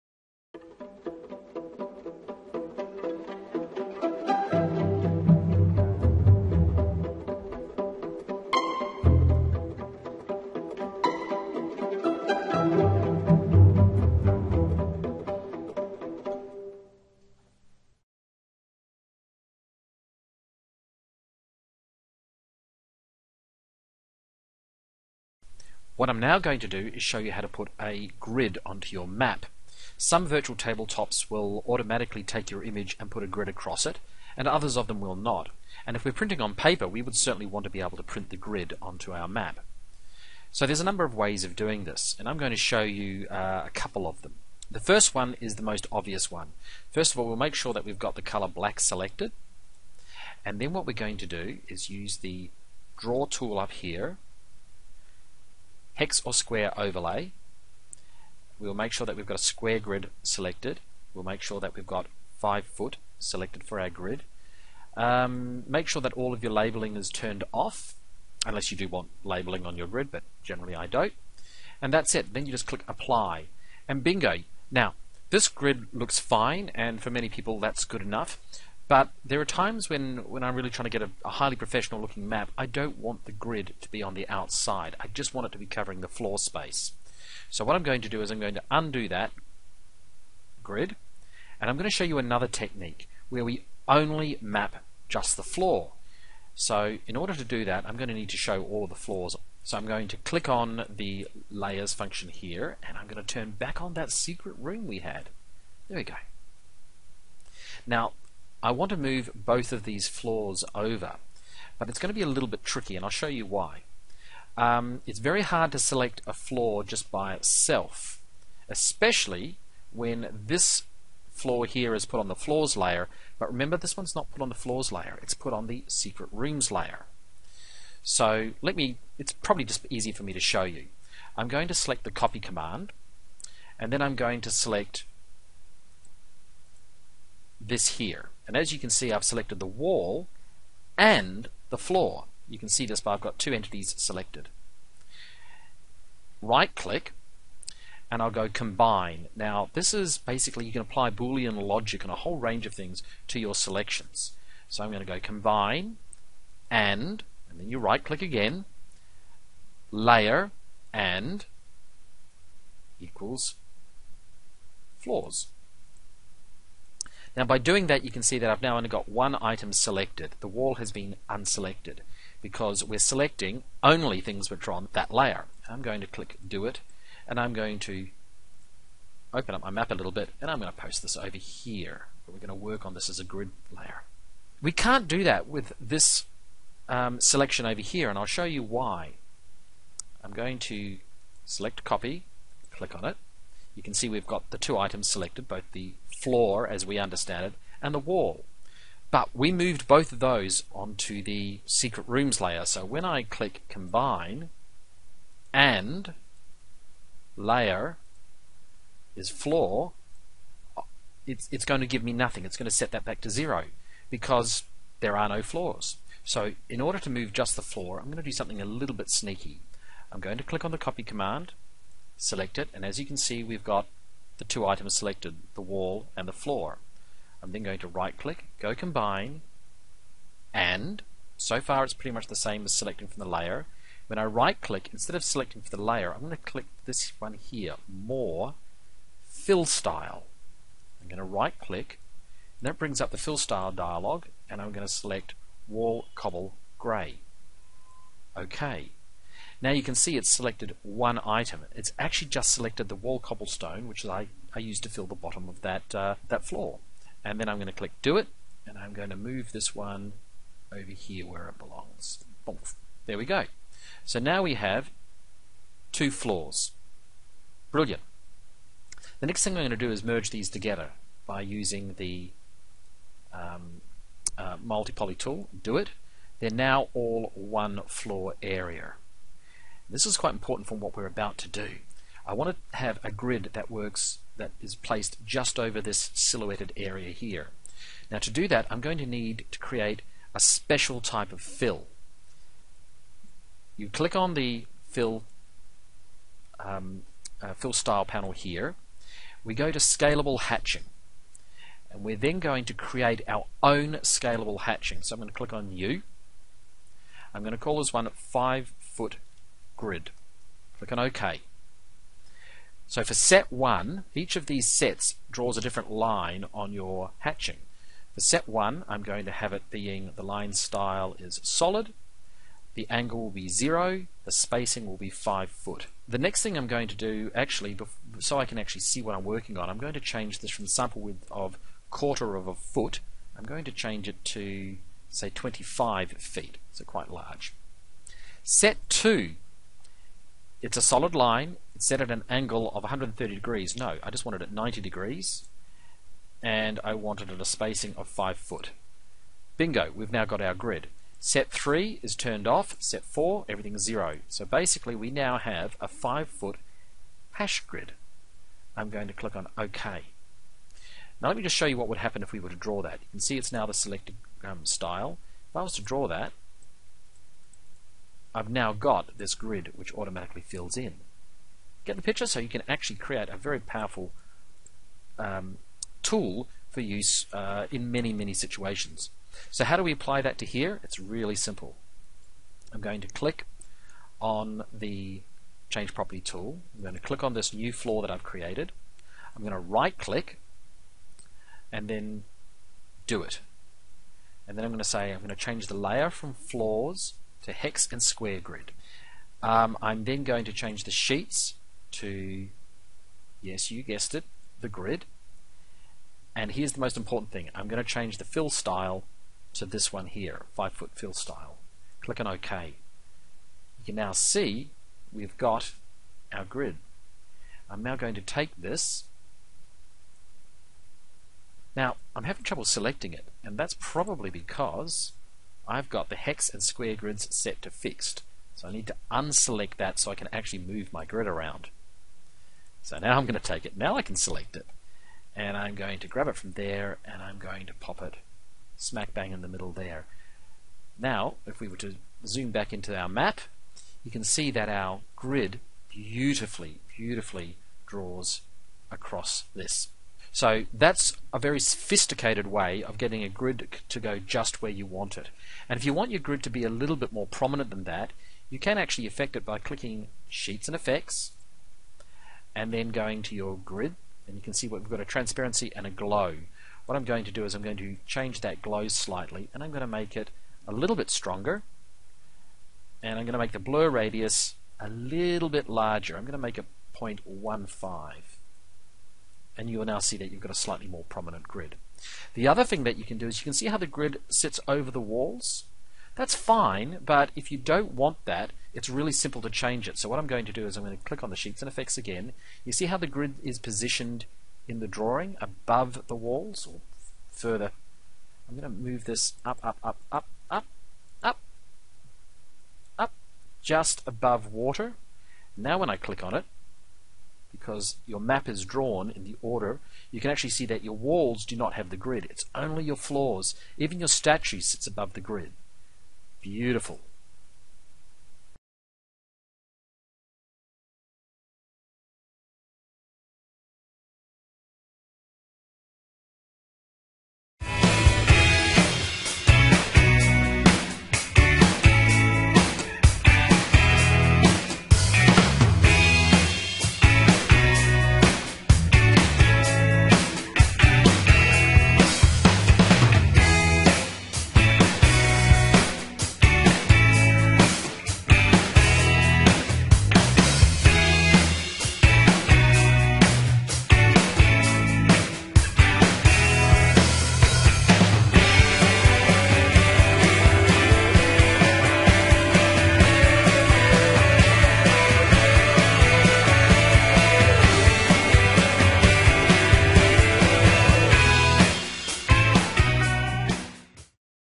Are Grids a Grind? Fret no more! Dungeon Mapping (Part 3)
In part three of your battlemap tutorial, you will learn not one, but two ways to place a grid over your map. The first takes seconds and places a grid over everything. The second way limits the grid to the floor of your map. You will also learn how to adjust the order of how things are drawn, powerful new ways to select objects in your map and new uses for the multipoly tool.